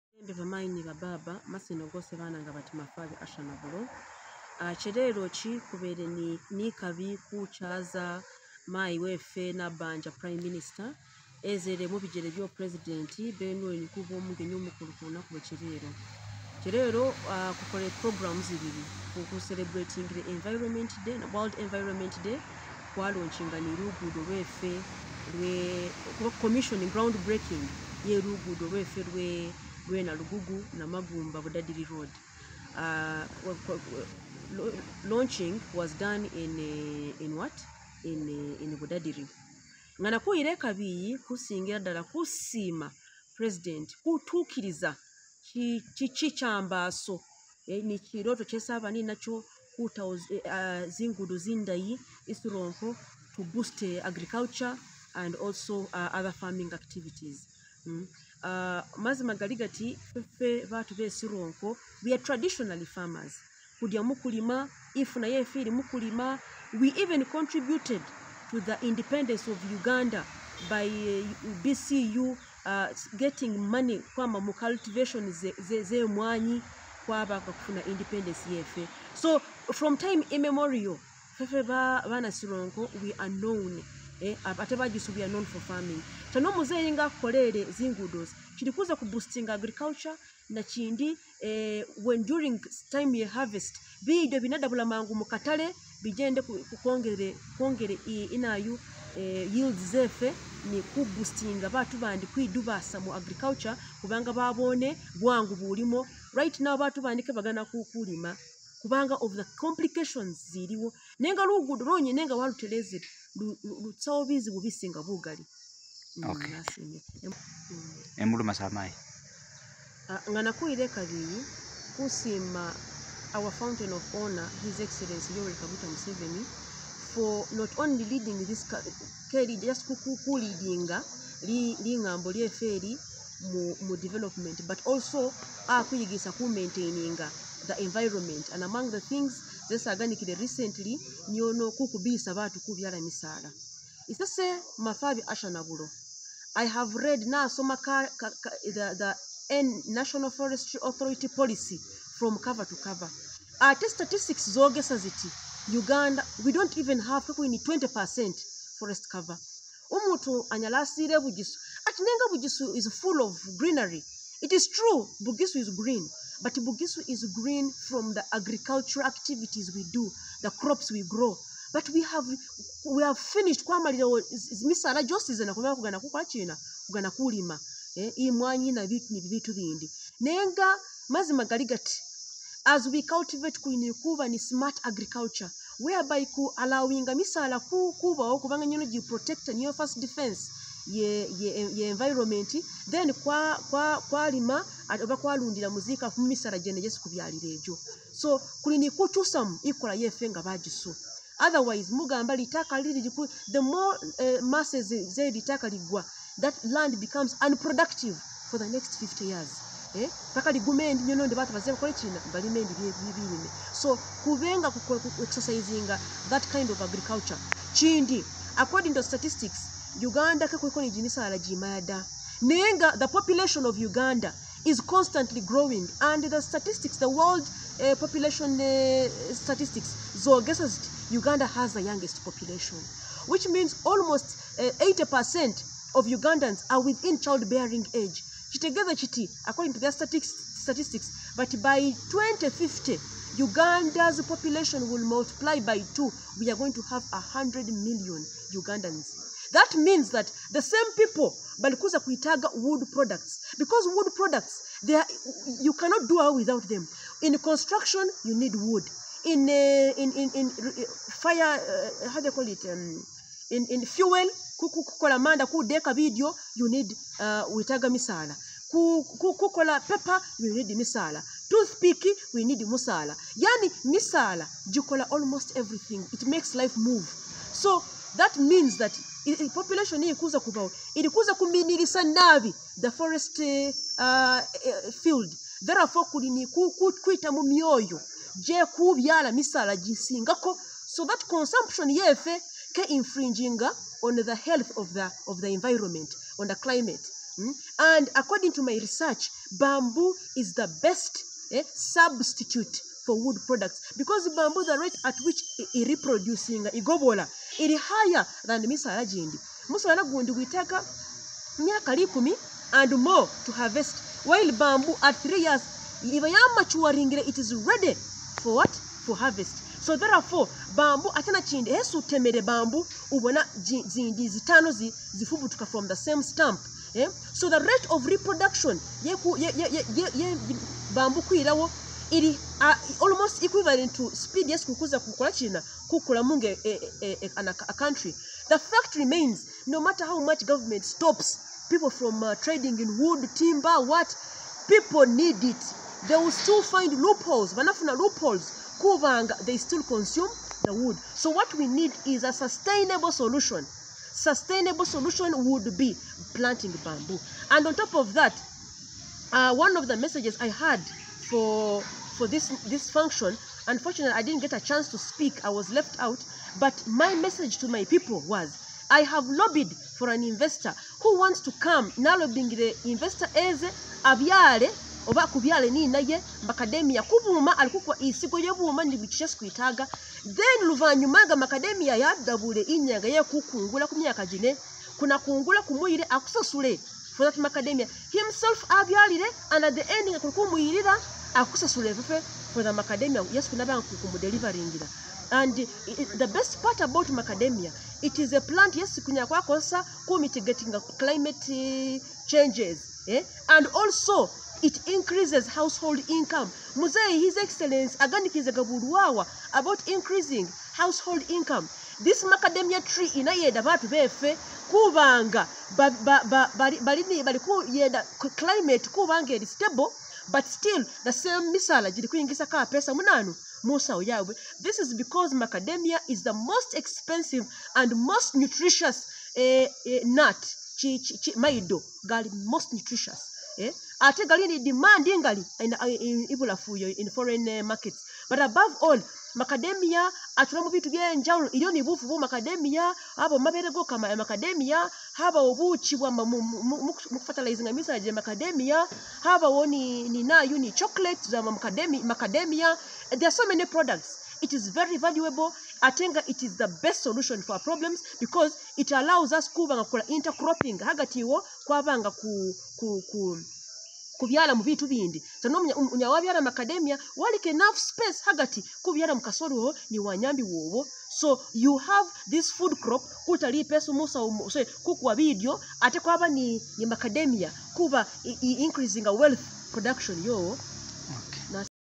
Sironko.Mayi Asha to donate Macadamia trees to improve on the Env't- her message during Env't day
President calls for tree planting to conserve the environment

By Weswa Ronnie

The president of Uganda Yoweri Kaguta Museveni has urged people to plant more trees as a way of conserving the environment.

International environmental day was organized by National Environment Management authority under the theme land restoration for climate resilience.

These  remarks were made by Robin Nambaja the prime minister of Uganda while reading the message of the president yesterday on Wednesday during international  environmental day celebration which was held at Mafudu primary school play ground located at Mafudu sub county in Sironko district.

According to Museveni, Uganda is experiencing climatic changes every day due to people continuing cutting down of trees and encroachment on natural resources.

He says that people must embrace tree planting in their areas as away of averting such climatical changes.

Beatrice Anywar the Minister of environment says that  government  under national forestry authority NFA has embarked on distributing free bamboo seedlings to districts of mountainous areas.

She says that they want bamboo to be planted and held soil to avoid land slides and flash floods.

Asha Nabulo Lumolo the former Sironko district woman member of parliament constant on NRM ticket welcomed the president's message saying in twenty years coming the country specially Bugisu region are going to be affected with hanger due to drought caused by cutting down of trees.

Lumolo adds that she has a plan of supplying academia tree species seedlings in this year to people of Sironko as away of improving on trees planting and environmental conservation.

She urged district environmental offices to start sensitizing the public about dangers of cutting down trees without replacement and destroying the wetlands to save the environment.

Nathan Nandala Mafabi member of parliament representing Budadiri West constituency in Sironko district says that people are cutting down trees for fuel do to expensiveness of electricity and gass they can use for cooking.

He urged government to cut down the prices of electricity and gass such that people can leave the habit of cutting down trees.